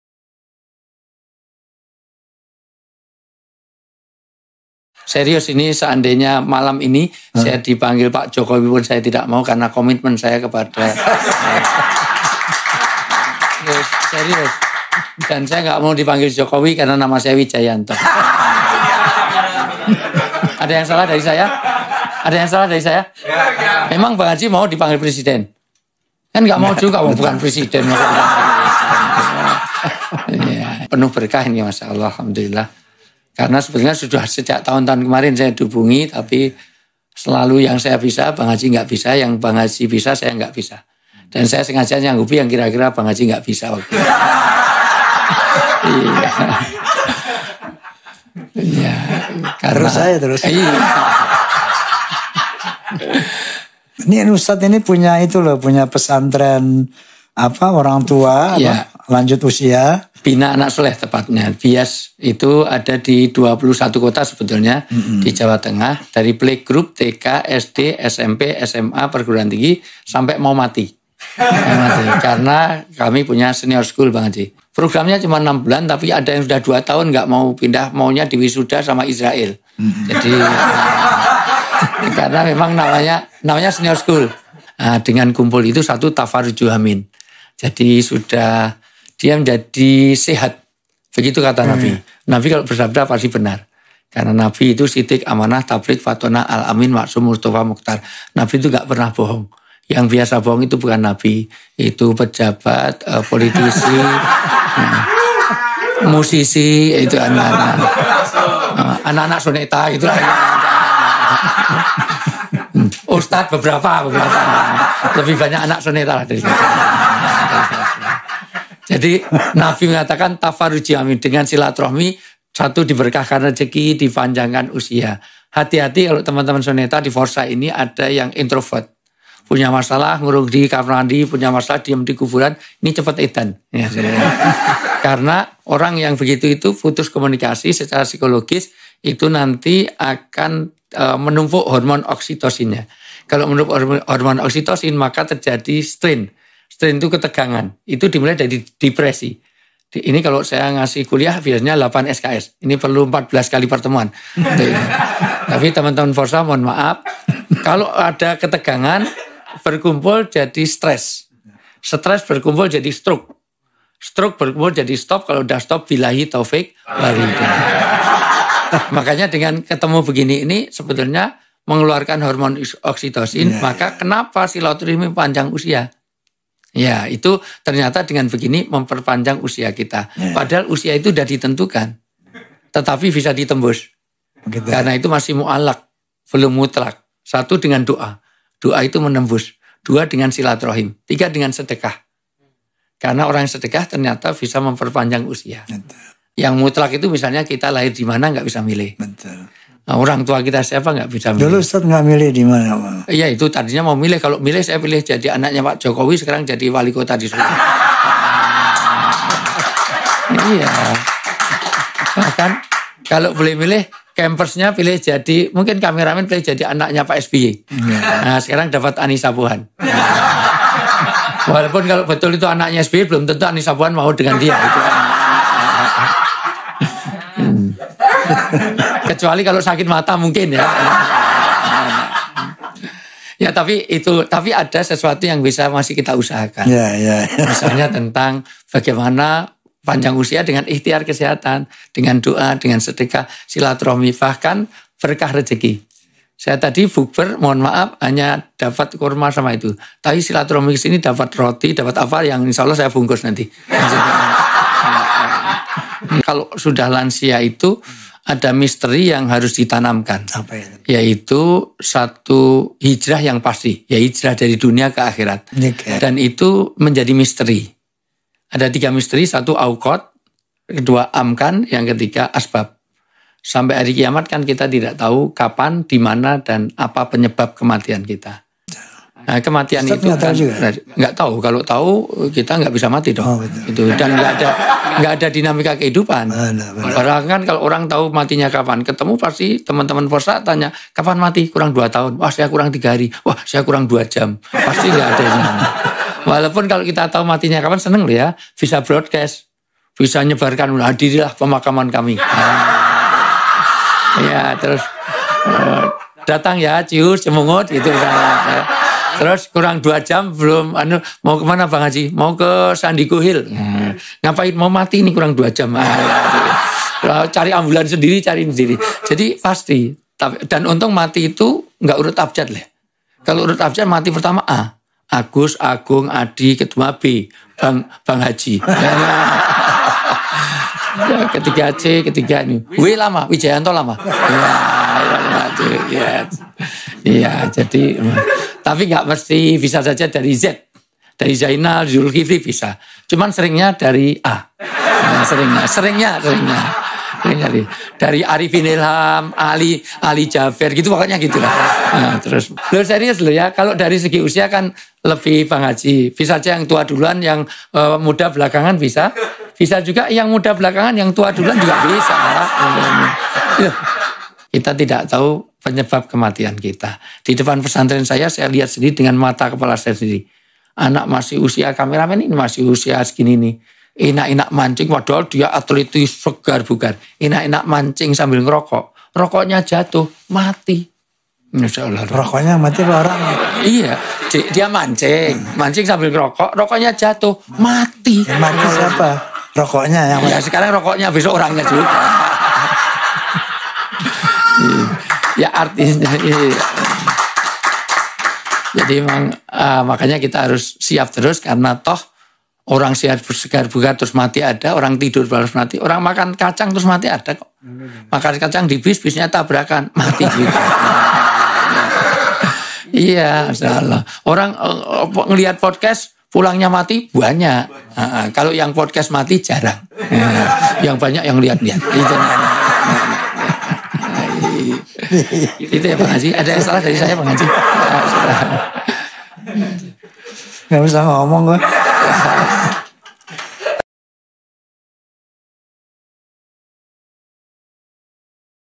CANDAAN USTAD WIJAYANTO
Candaan segar namun syarat makna ketika diwawancarai H Rhoma Irama semoga menghibur.